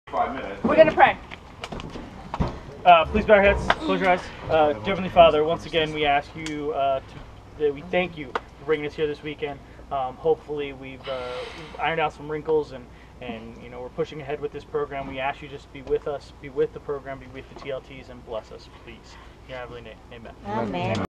Potomac Conference TLT Introduction Part 7 GOPR1044
This was a Teen Leadership in Training introduction done by Jenny and James during the Potomac Conference Leadership weekend at Camp Blue Ridge in Montebello, Virginia.